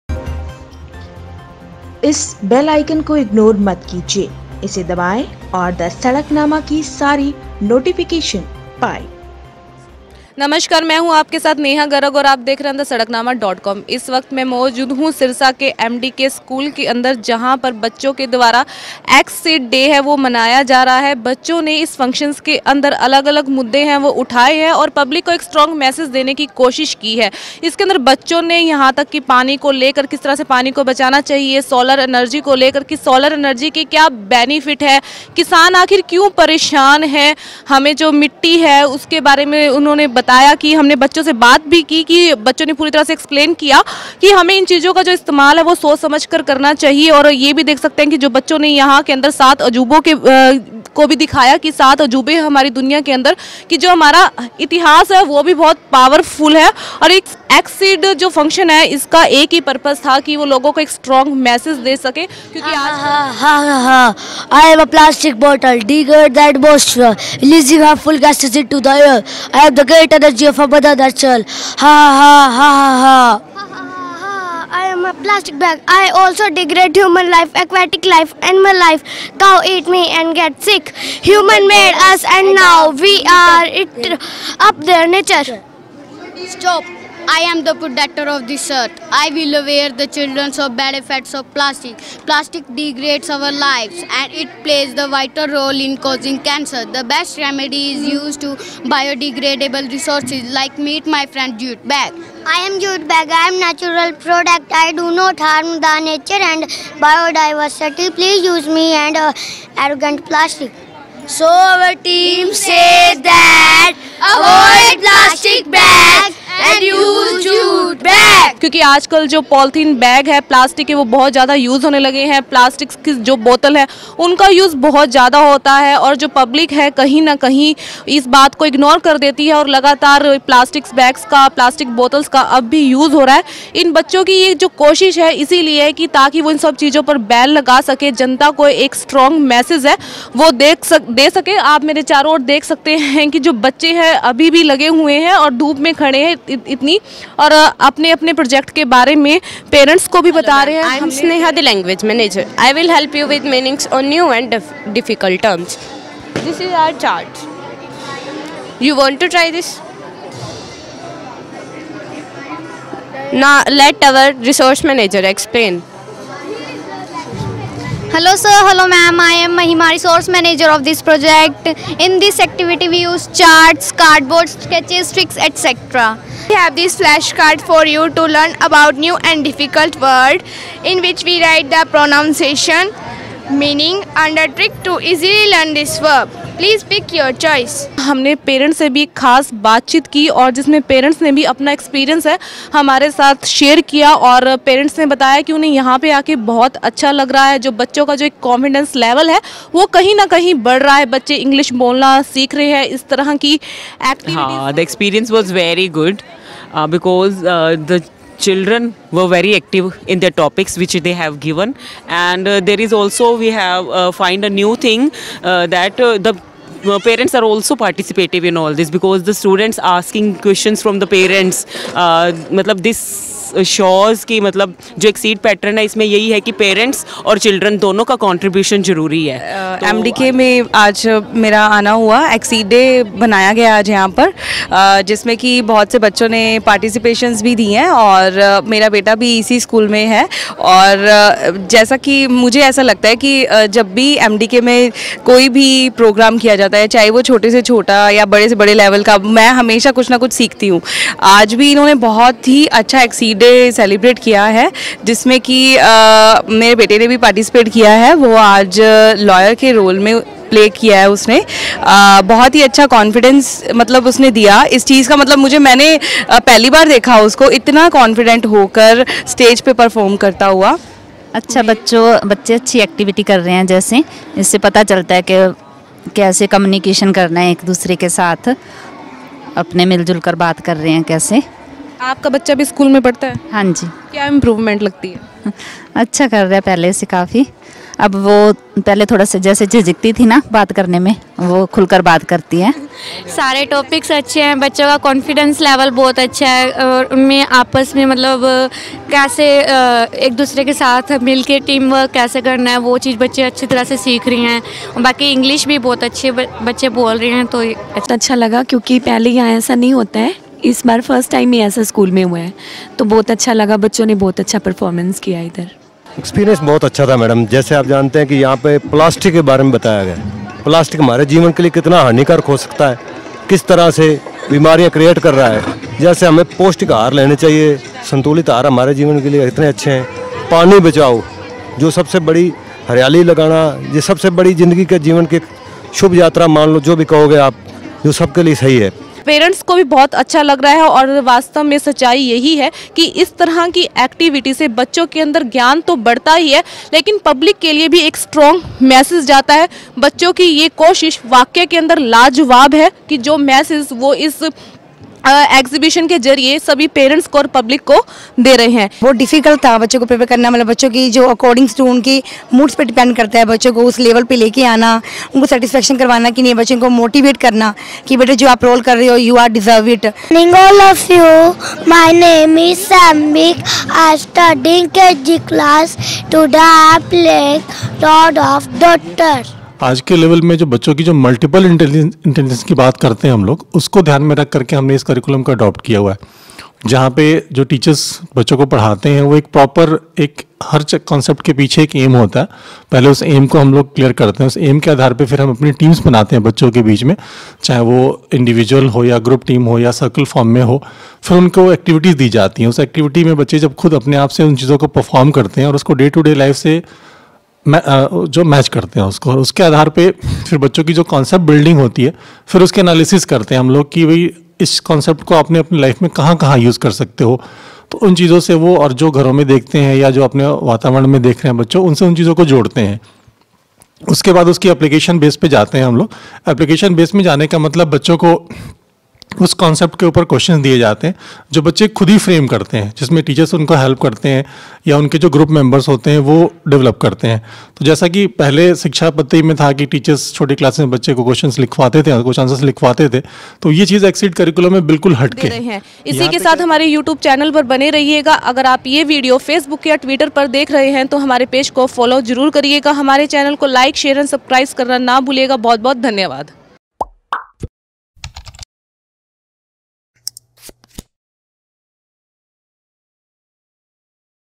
एमडीके स्कूल के बच्चों ने प्रदर्शनी में दिखाया मॉर्डन खेती का मास्टर प्लान, आप भी जानिए
हरियाणा के सिरसा के एमडीके इंटरनेशनल स्कूल में प्रदर्शनी आयोजित, स्कूली बच्चों ने प्लास्टिक से लेकर मॉर्डन खेती और ग्रीन हाउस तकनीक को समझाया, सूक्ष्म सिंचाई प्रणाली और अक्षय ऊर्जा को लेकर भी कई मॉडल दर्शाये गए, टीचर से लेकर अभिभावक ने की तारीफ, देखिये ये रिपोर्ट नेहा गर्ग के साथ वीडियो जर्नलिस्ट चरण सिंह।